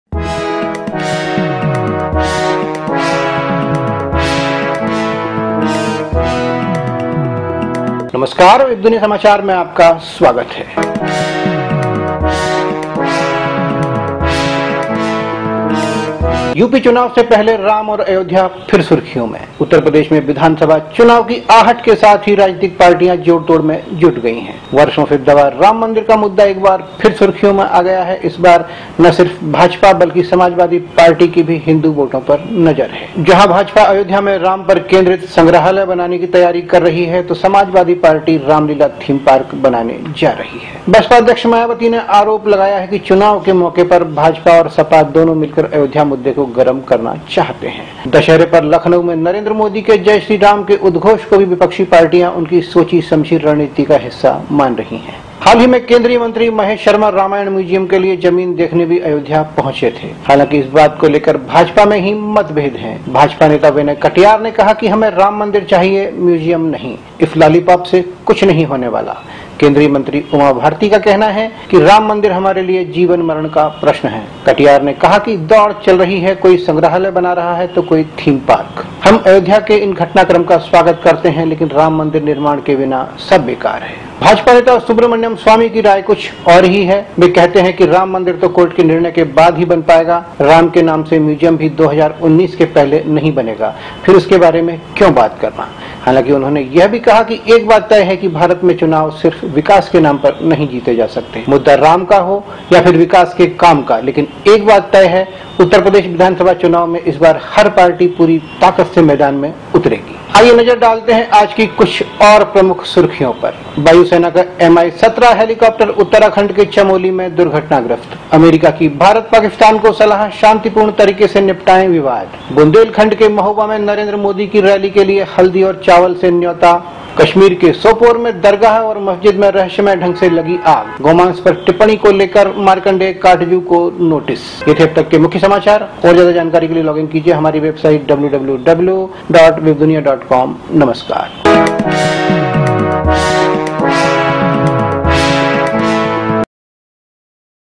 यूपी चुनाव से पहले राम और अयोध्या फिर सुर्खियों में
उत्तर प्रदेश में विधानसभा चुनाव की आहट के साथ ही राजनीतिक पार्टियां जोड़तोड़ में जुट गई हैं। वर्षों से दबा राम मंदिर का मुद्दा एक बार फिर सुर्खियों में आ गया है। इस बार न सिर्फ भाजपा बल्कि समाजवादी पार्टी की भी हिन्दू वोटों पर नजर है। जहां भाजपा अयोध्या में राम पर केन्द्रित संग्रहालय बनाने की तैयारी कर रही है तो समाजवादी पार्टी रामलीला थीमपार्क बनाने जा रही है। बसपा अध्यक्ष मायावती ने आरोप लगाया है कि चुनाव के मौके पर भाजपा और सपा दोनों मिलकर अयोध्या मुद्दे को गरम करना चाहते हैं।